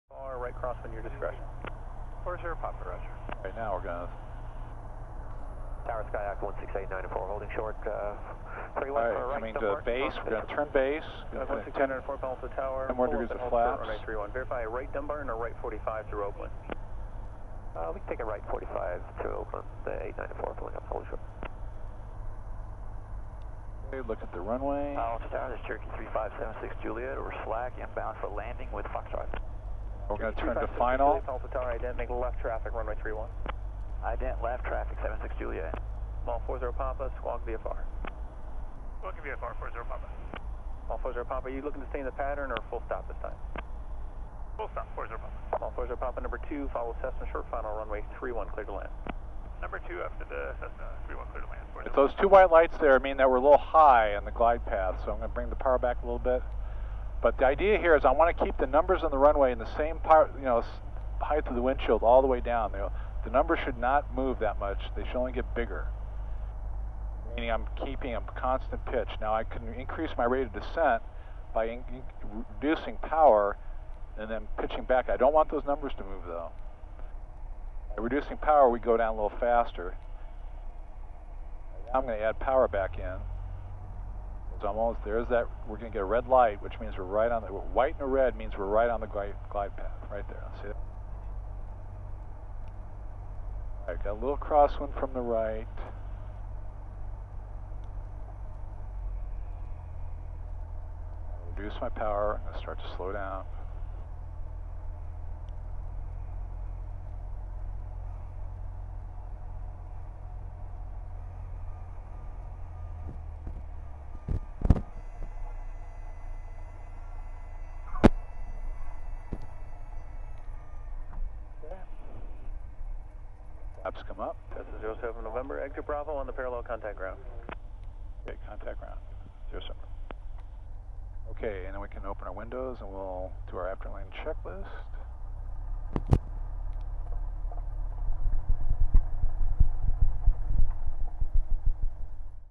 Stabilized approach to landing - KPAO
The key to performing consistently good landings in a Cessna 172 is maintaining a stabilized approach (65 kts, on glide path) immediately after turning from base to final.  Once the proper pitch is established for 65 kts, that pitch should not vary at all regardless of power setting.  Meanwhile, power is increased or reduced according to what is required to achieve the rate of descent desired to match the proper glide path.